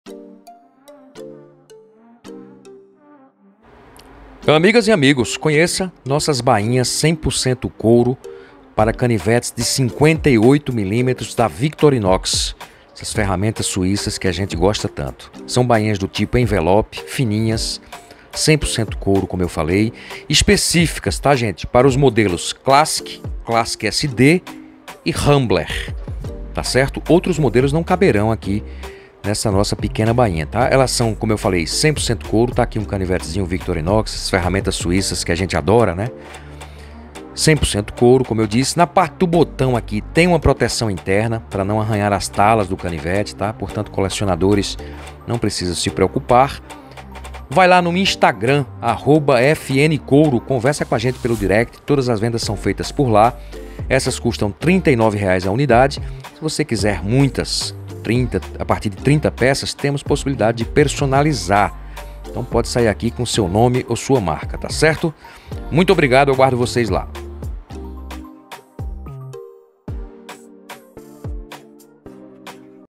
Bainhas para canivetes Victorinox 58 milímetros.
BAINHAS PARA CANIVETES VICTORINOX 58MM, COURO BOVINO PURO.

Canivete Victorinox 58 milímetros.  Bainhas 100% couro puro para canivetes victorinox 58mm. Exclusivas para os modelos: Classic, Classic SD e Rambler. Canivete Victorinox Classic. Classic SD. Rambler.

Custam R$ 39,00
Enviamos para todo Brasil via Coreeios PAC.
Todas as vendas pelo direct no Instagram de nossa pequena Lojinha:  @fncouro

A partir de 30 unidades é possível encomendar e personalizar com seu nome ou até sua marca.